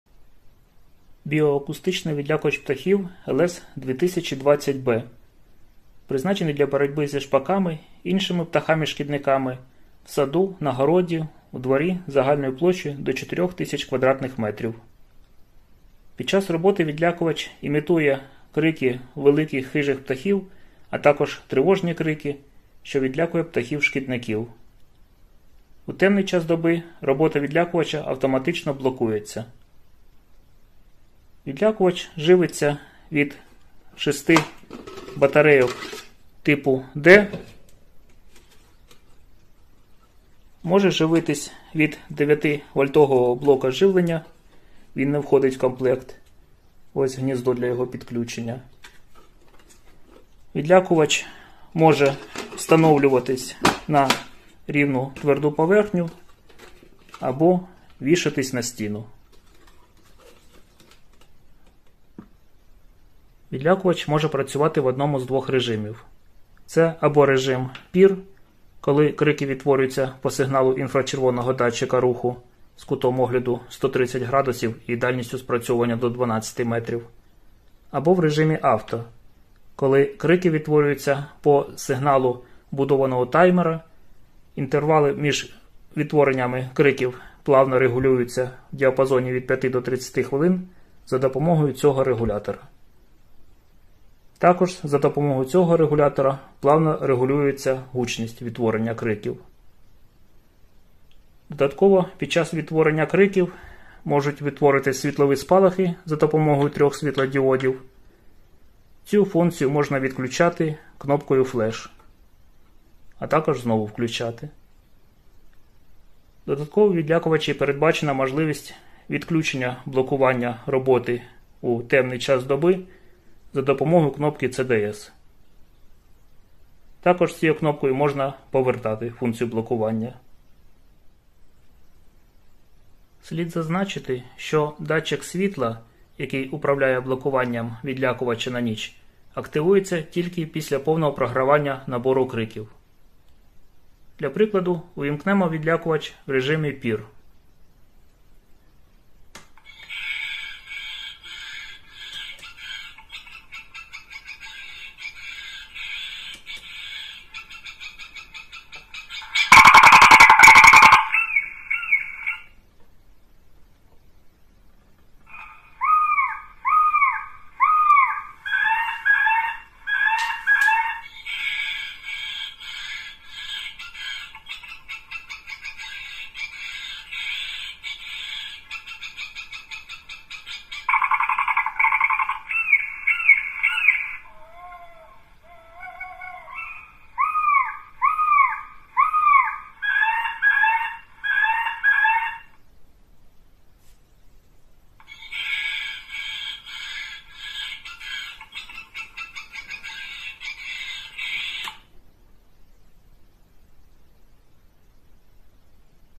LS-2020B - Звуковий відлякувач птахів на батарейках (до 4000 м2)
Біоакустичний відлякувач птахів Leaven LS-2020B для відлякування шпаків, ластівок, сусідських курей в саду, на городі, ділянці, у дворі площею до 4000 м2. Захист черешні від шпаків. Відеоогляд.
Биоакустический отпугиватель птиц для отпугивания скворцов, ласточек, соседских курей в саду, на огороде, участке, во дворе. Защита черешни от скворцов. Видеообзор.